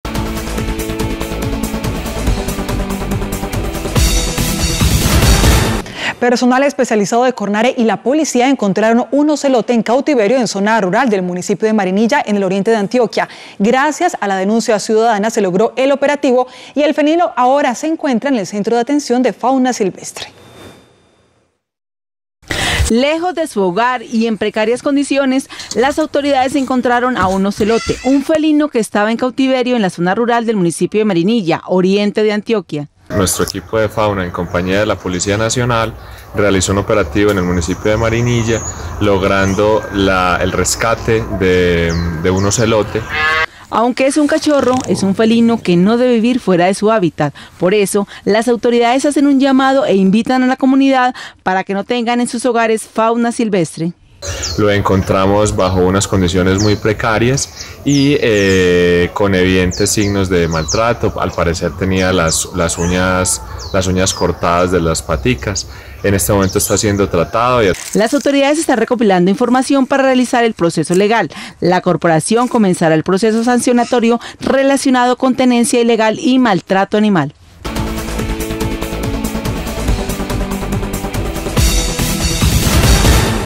Rescatan Ocelote en Marinilla - Teleantioquia Noticias
Personal especializado de la Policía y Cornare, encontraron un ocelote. Un felino que se hallaba en cautiverio en zona rural del municipio de Marinilla, Oriente de Antioquia. Gracias a la denuncia ciudadana, se logró el operativo. El animal está en el Centro de Atención de Fauna Silvestre de Cornare.